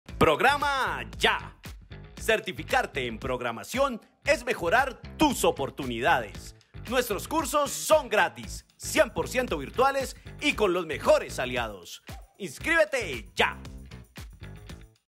💻 Cursos de programación Gratis ProgramaYa 100% Virtual
¡INSCRÍBETE Y CERTIFÍCATE EN HABILIDADES DE PROGRAMACIÓN!

💻 REQUISITOS:
Para participar se debe cumplir con los requisitos exigidos en su totalidad, revisar el calendario, preparar la información y los documentos que se exigen para la inscripción.

Los interesados en participar en la presente convocatoria deberán cumplir y acreditar la siguiente información:

Tener nacionalidad colombiana
Ser mayor de 14 años (los mayores de 14 y menores de 18 años deberán ser inscritos por alguno de sus padres, o tutor o acudiente).
Diligenciar el formulario de inscripción en:
Tener una cuenta propia y personal de correo electrónico.
Tener disponibilidad para el desarrollo de la formación de hasta 15 horas semanales las cuales serán distribuidas en sesiones sincrónicas y asincrónicas por el operador de la formación.
Leer los requisitos de la convocatoria y asegurarse de cumplirlos en su totalidad.
Diligenciar la totalidad de los campos solicitados en el formulario de inscripción, así como adjuntar la documentación requerida.
Estar atentos al calendario.